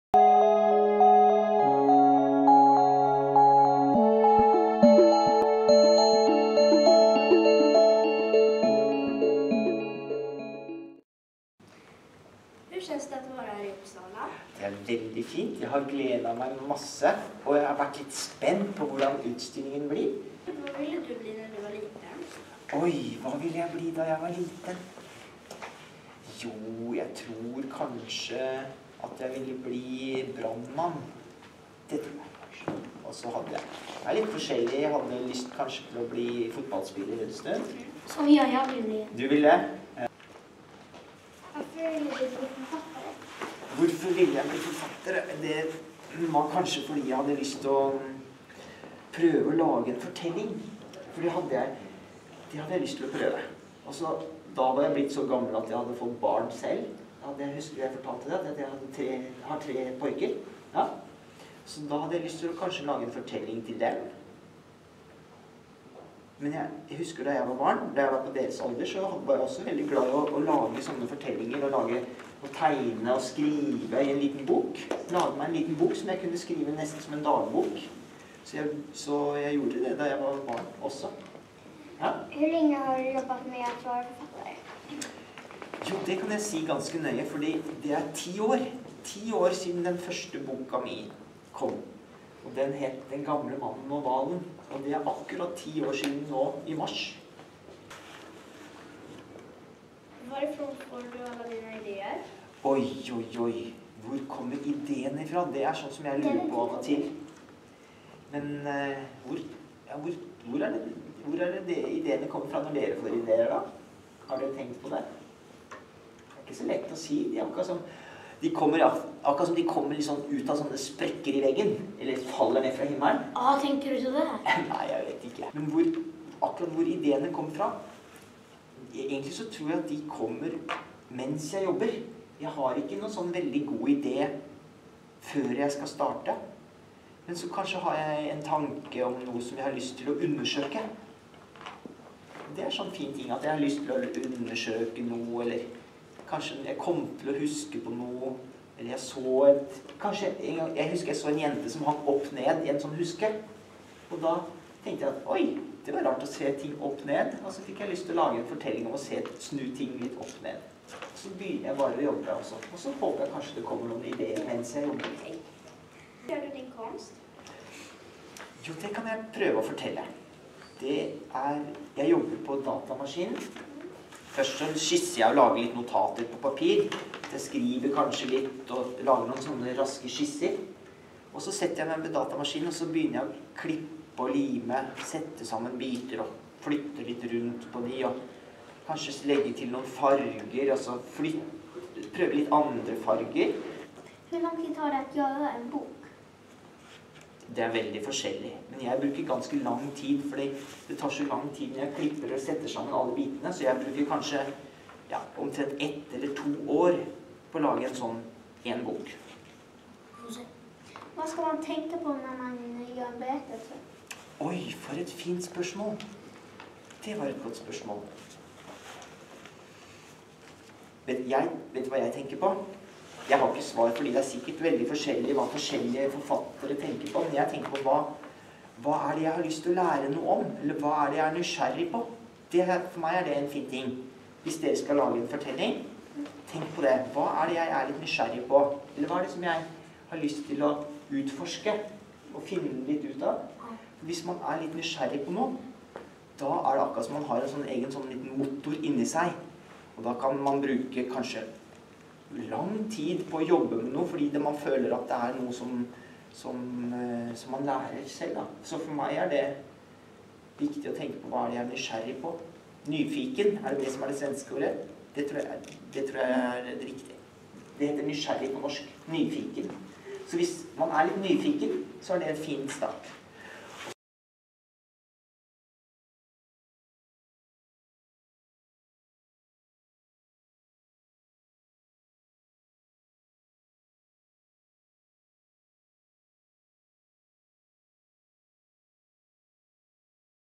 Frågvisa intervjuar: Stian Hole, korta versionen
Elever från år 3 på Växthuset intervjuar Stian Hole på Bror Hjorths hus inför utställningen Mellan dröm och verklighet våren 2015.  Illustrationerna i början och slutet är gjorda av Stian Hole och finns som en vägg i utställningen. Se även